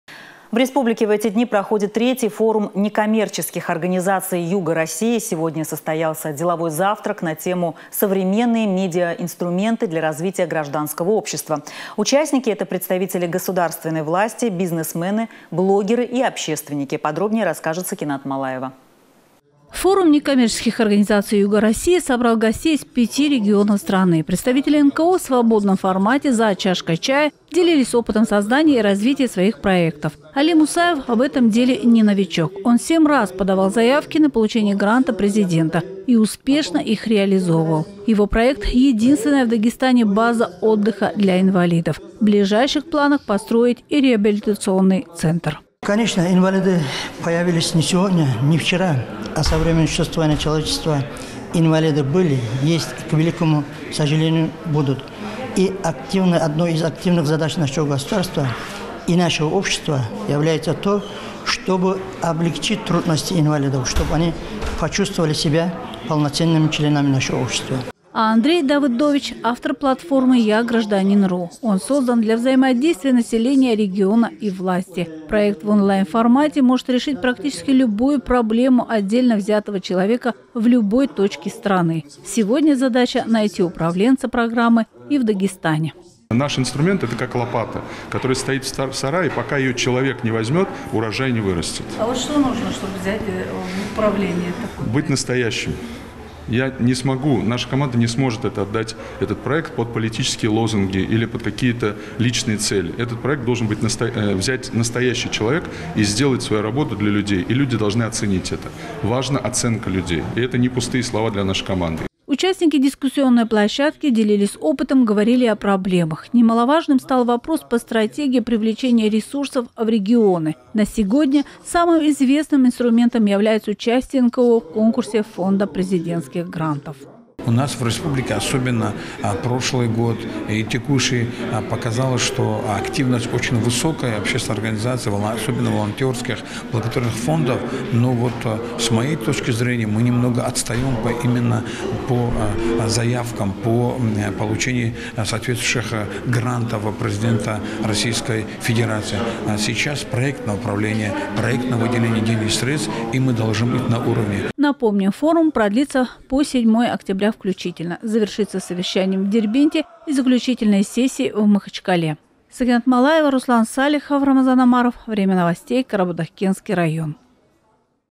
В Дагестане проходит Форум НКО Юга России
В республике проходит III форум некоммерческих организаций Юга России. Сегодня состоялся деловой завтрак на тему «Современные медиа-инструменты для развития гражданского общества». Участники –  представители государственной власти, бизнесмены, блогеры и общественники.